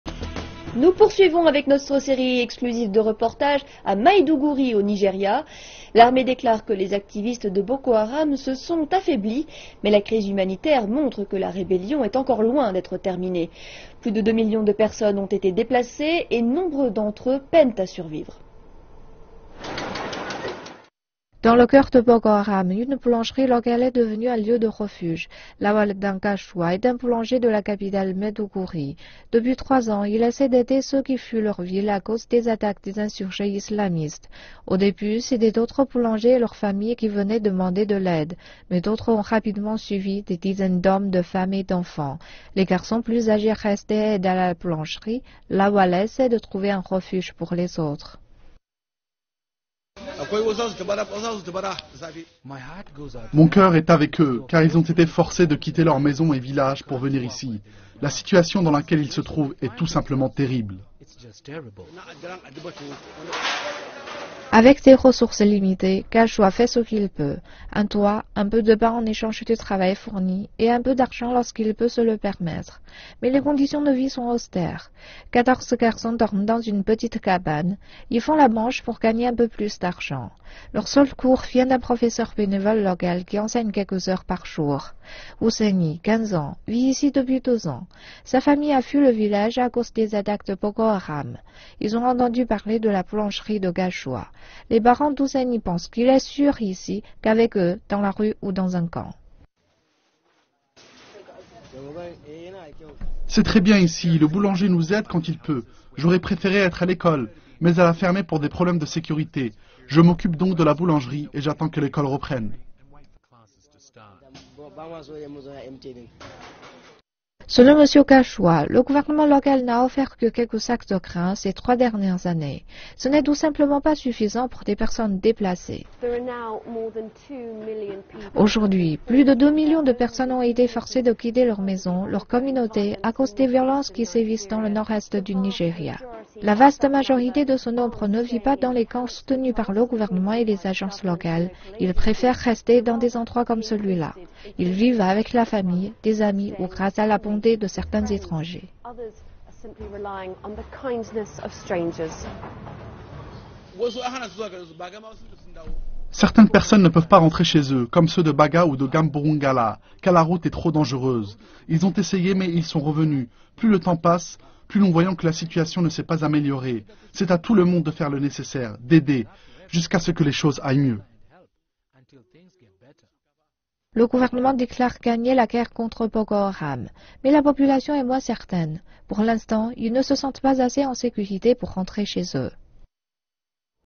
Un boulanger local vient en aide aux personnes déplacées
Nous poursuivons avec notre série exclusive de reportages à Maiduguri au Nigeria. L'armée déclare que les activistes de Boko Haram se sont affaiblis. Mais la crise humanitaire montre que la rébellion est loin d'être terminée. Plus de 2 millions de personnes ont été déplacées et nombreux d'entre eux peinent à survivre.